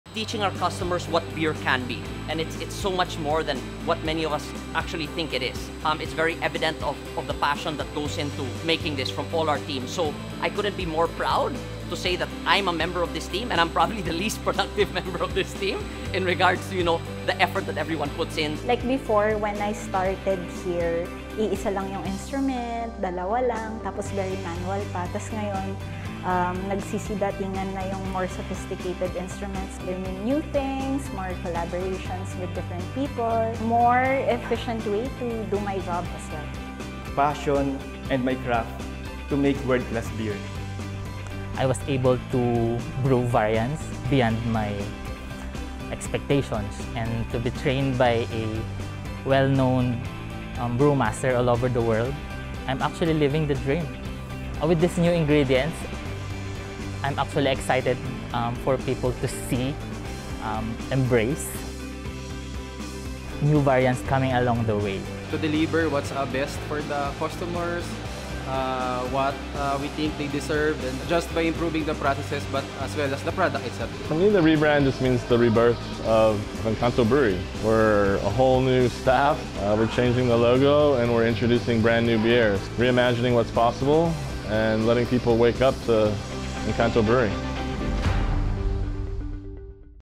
Behind the Rebrand: Personal Perspectives from Team Engkanto
During Engkanto’s rebrand a few months back, excitement filled the brewery☀️ And a lot of expectations. What does these mean to the people behind our great craft beers?